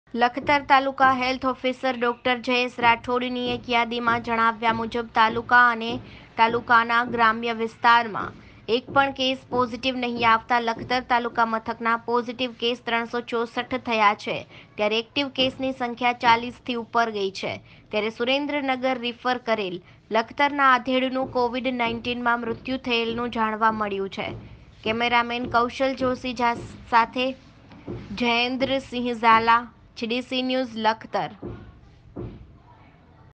લખતર તાલુકા મથક અને ગ્રામ્ય વિસ્તારમાં એકપણ કેસ કોરોના પોઝીટીવ નહિ આવતા તંત્ર એ રાહત નો શ્વાસ લીધો
કોરોના શરદી ઉધરસ ને લગતો રોગ હોય સાવચેતી રાખવી અત્યંત જરૂરી